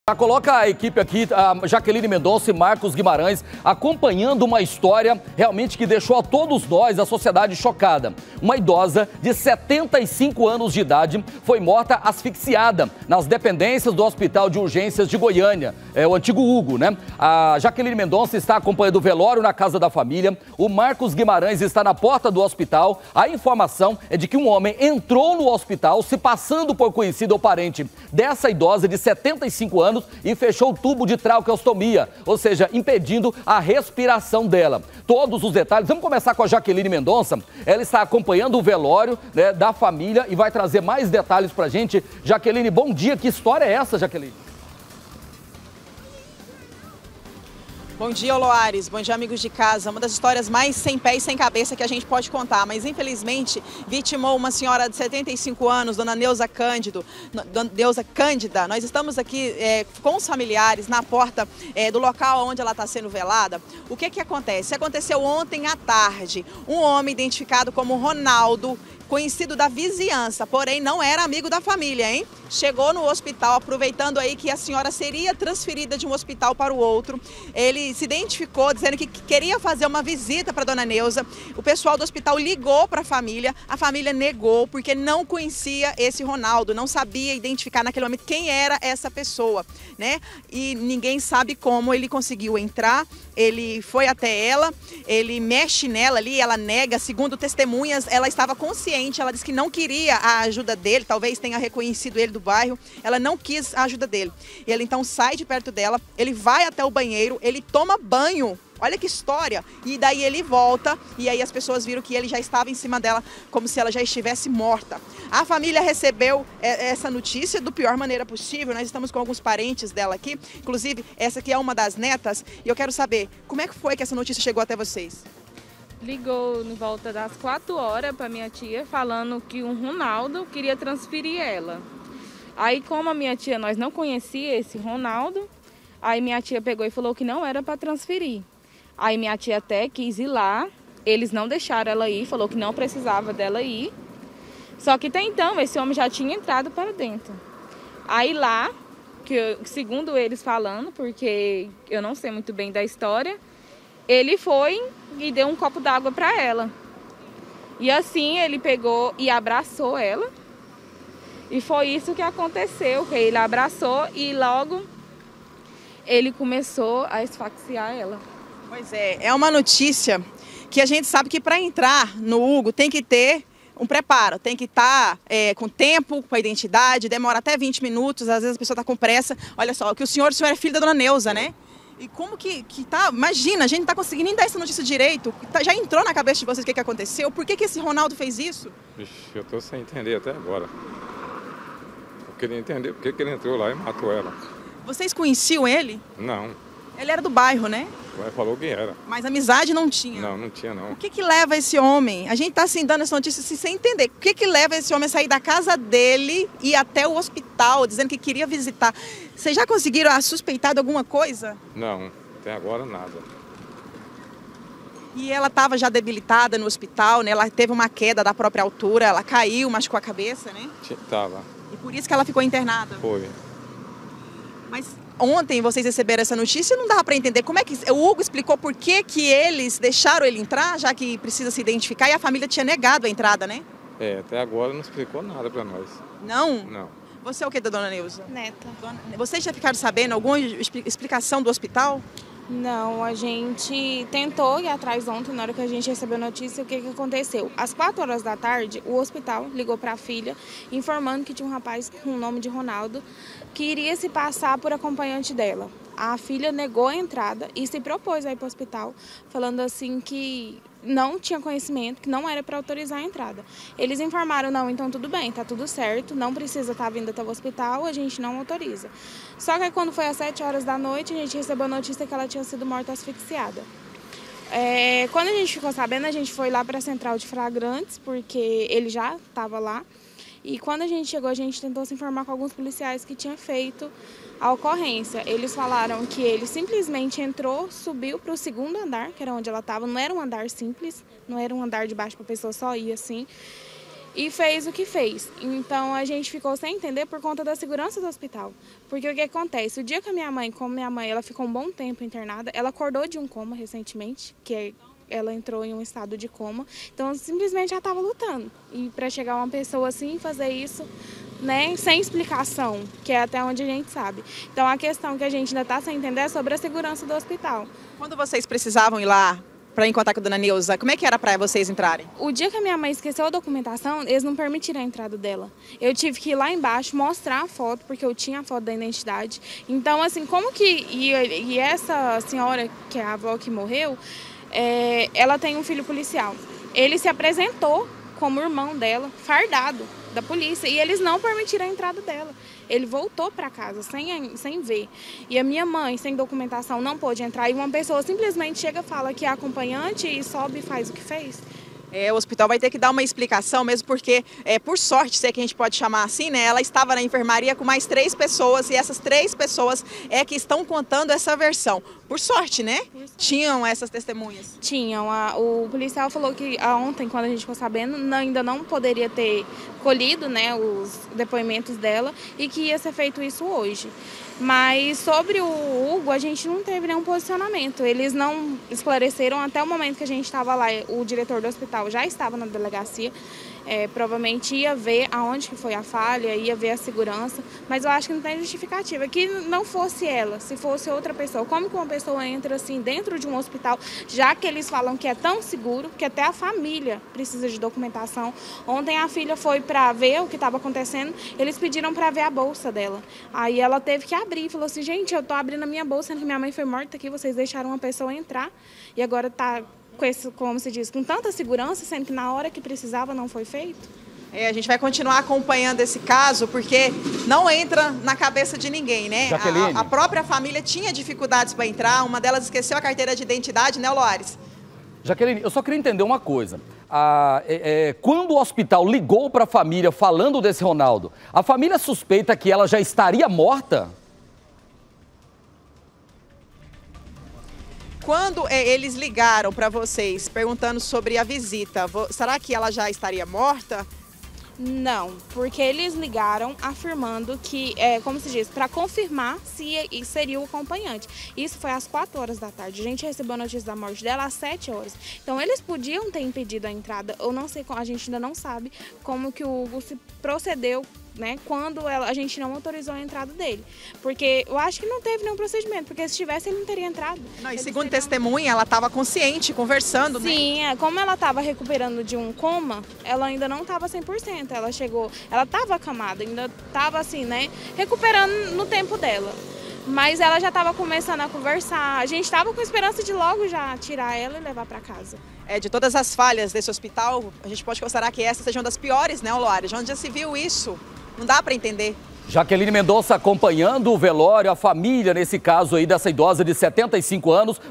CRIME BÁRBARO: IDOSA É ASSASSINADA DENTRO DO HUGO
IDOSA DE 75 ANOS É MORTA ASFIXIADA NO HOSPITAL ESTADUAL DE URGÊNCIAS DE GOIÂNIA.